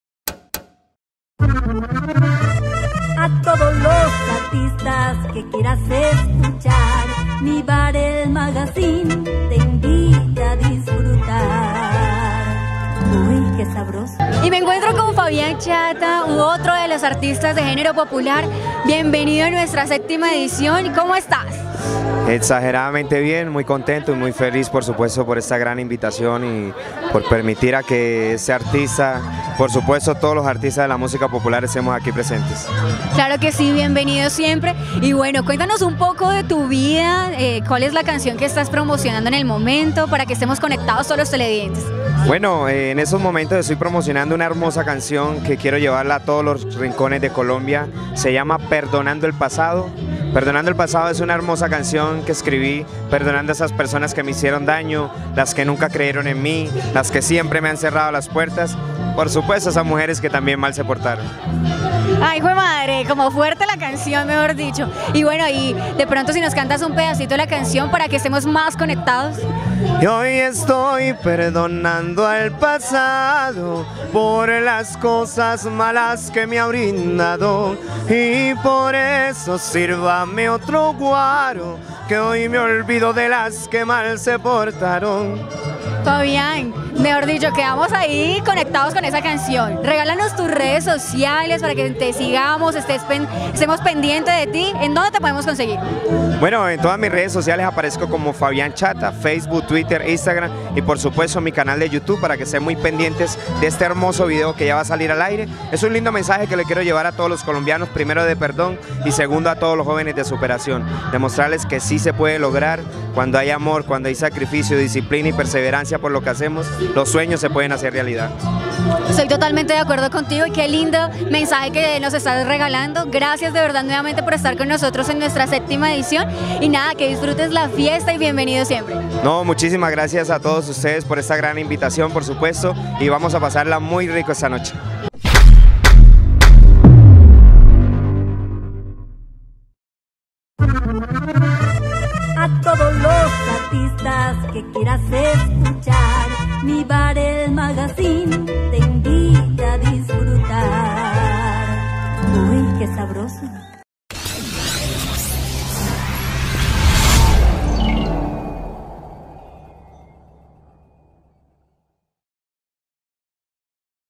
Fabian Chata en el Lanzamiento de la Septima edicion
Artista invitado al Lanzamiento de la Séptima edición de la Revista Mi Bar el Magazine!!